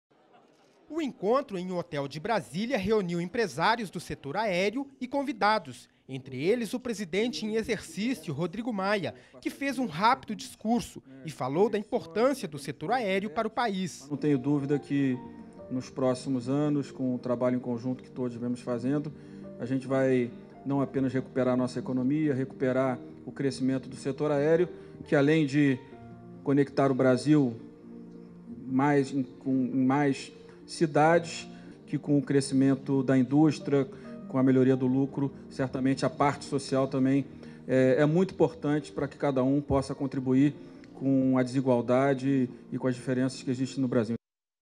Presidente em exercício participa do Conexões Transformadoras
REPÓRTER NBR - 30.08.17: Presidente em exercício, Rodrigo Maia, participa do Evento Conexões Transformadoras, que comemora os cinco anos da Associação Brasileira das Empresas Aéreas (ABEAR).